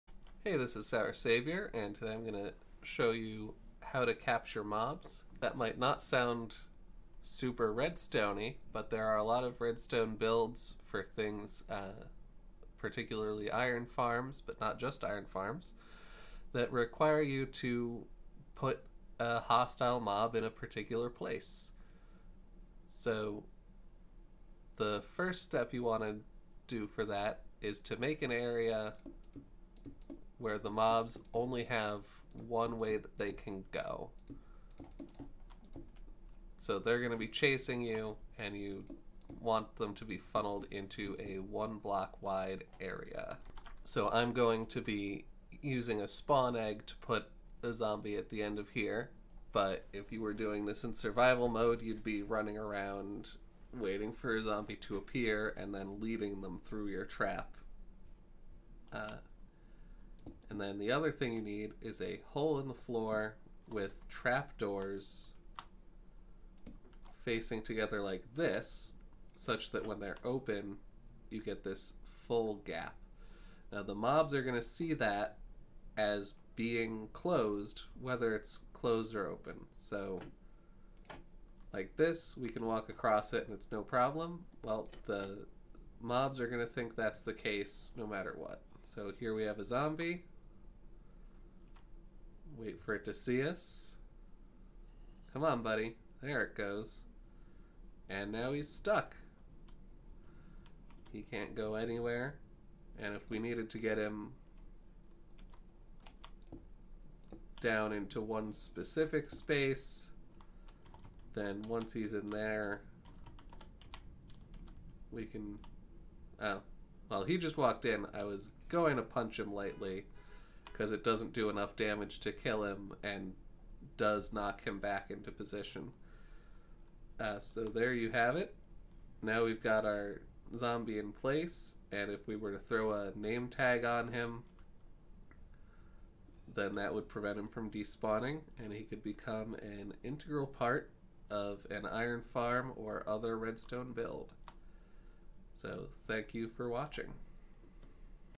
Redstone Basics: Capturing Hostile Mobs
Capturing mobs? How is that redstone? Certain automated farms require mobs to be in a particular place for them to work. In particular, iron farms tend to use zombies to spook villagers into making golems more frequently than the rumors method. I just built a half size version of the iron farm from TangoTek's Hermitcraft season 7 episode 2 this week (check out his channel if you want to see it, then mentally remove the second layer of modules) and part of that was getting a pair of zombies into position.

Or, if none of that justifies it for you, the creative menu lists trap doors as a redstone component.